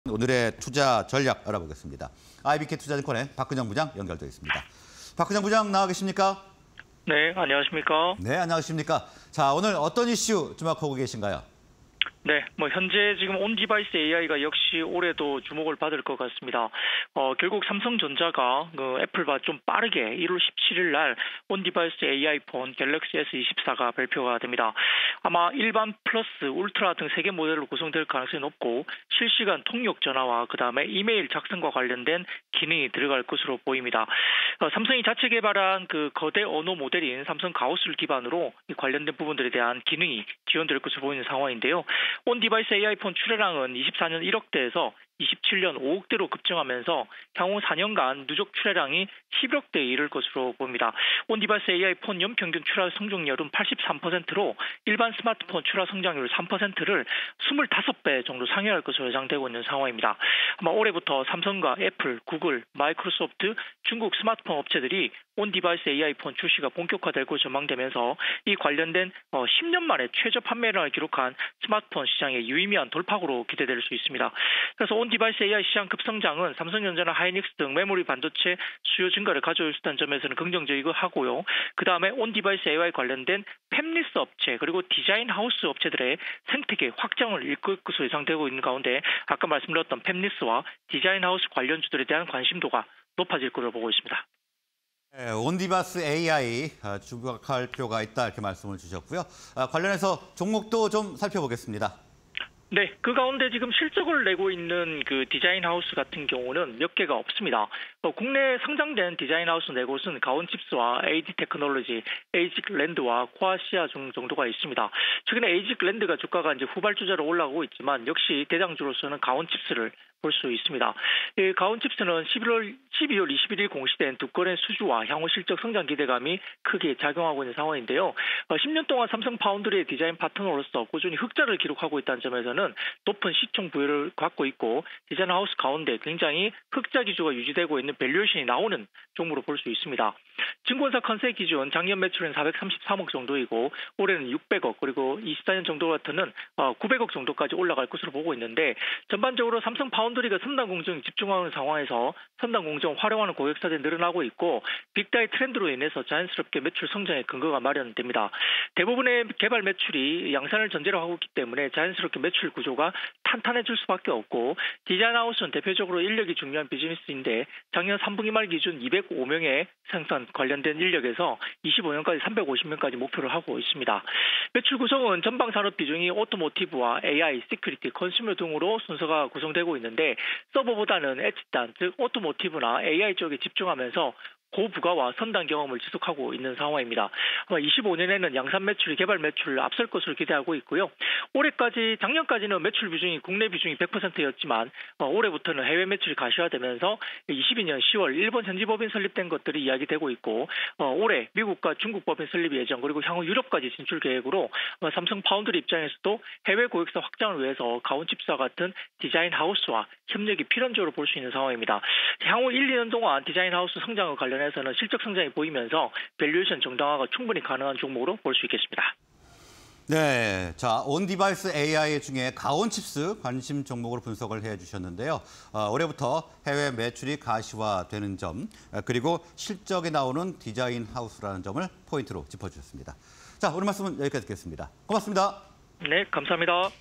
가온칩스(399720)... "갤럭시S24 초읽기"... 온디바이스 AI 수혜주는?_투자 인사이트 (20240104)
Q. "갤럭시S24 초읽기"... 온디바이스 AI 수혜주는?

가온칩스(399720)
- 안정적인 실적 및 수주 모멘텀 겸비
- 10년 삼성파운드리 디자인 파트너
- 매출 구조 탄탄... 내년 해외 매출 가시화